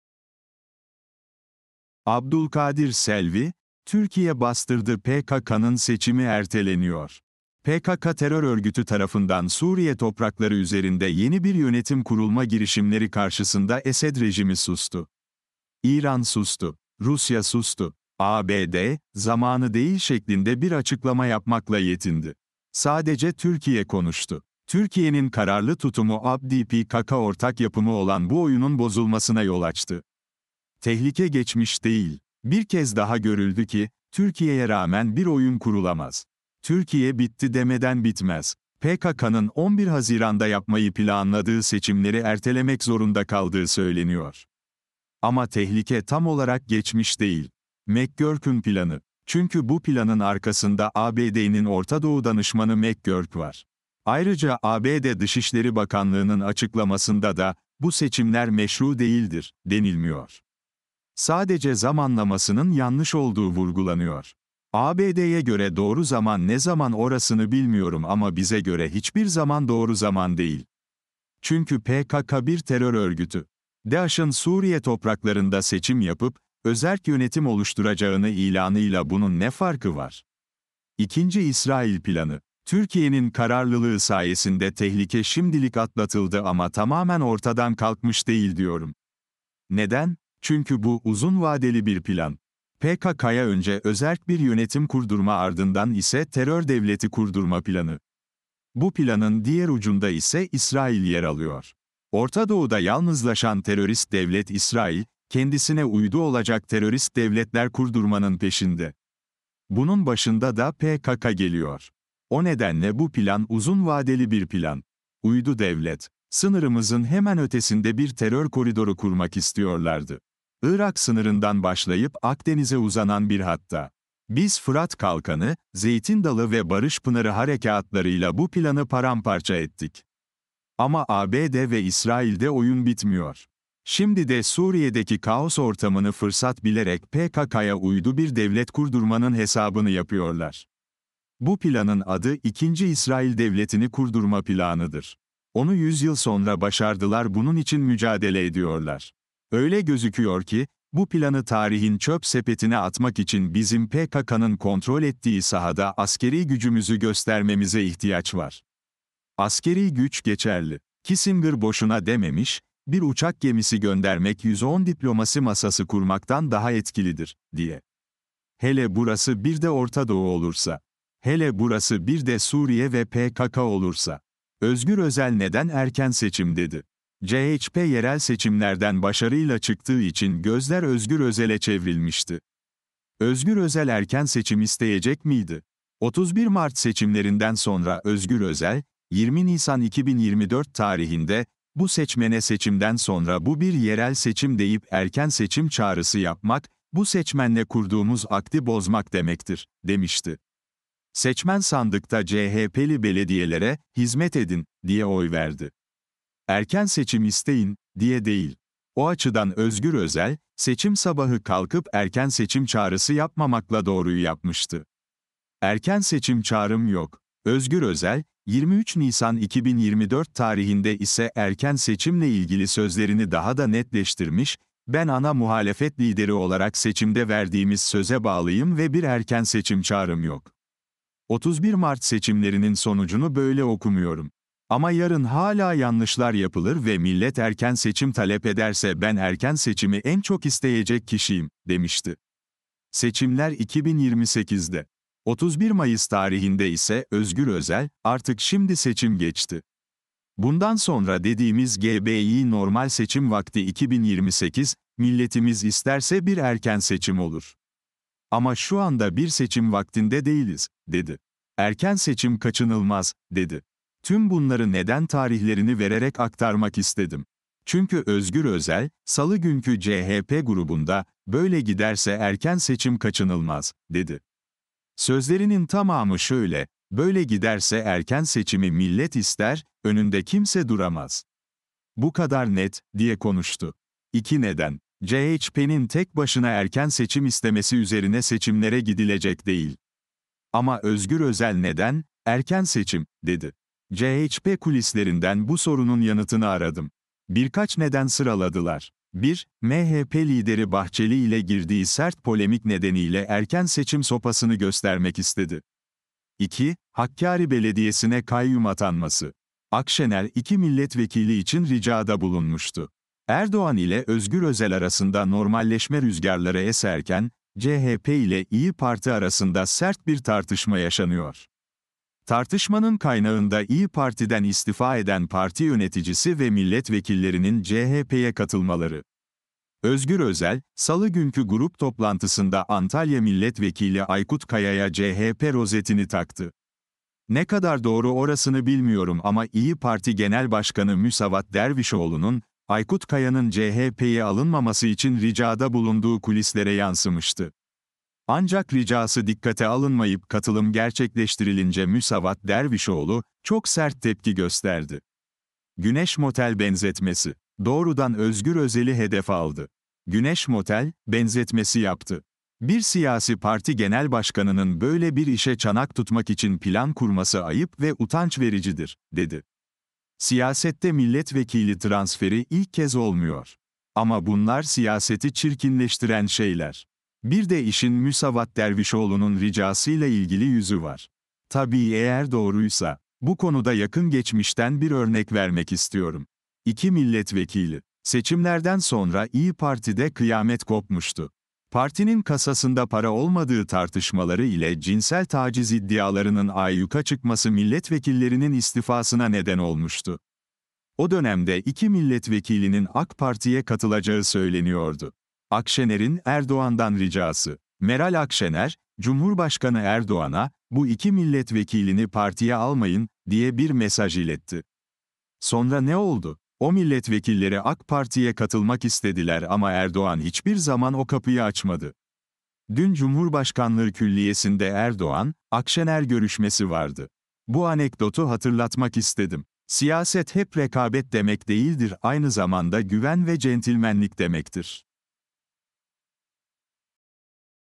ABDULKADİR SELVİ: TÜRKİYE BASTIRDI PKK’NIN SEÇİMİ ERTELENİYOR - SESLİ MAKALE DİNLE
Abdulkadir Selvi - hurriyet.com.tr - Türkiye bastırdı PKK’nın seçimi erteleniyor - 6 Haziran 2024 tarihli yazısının seslendirmesi

"PKK terör örgütü tarafından Suriye toprakları üzerinde yeni bir yönetim kurulma girişimleri karşısında Esed rejimi sustu. İran sustu. Rusya sustu. ABD, zamanı değil şeklinde bir açıklama yapmakla yetindi. Sadece Türkiye konuştu. Türkiye’nin kararlı tutumu ABD-PKK ortak yapımı olan bu oyunun bozulmasına yol açtı.

TEHLİKE GEÇMİŞ DEĞİL

Bir kez daha görüldü ki, Türkiye’ye rağmen bir oyun kurulamaz. Türkiye bitti demeden bitmez. PKK’nın 11 Haziran’da yapmayı planladığı seçimleri ertelemek zorunda k. "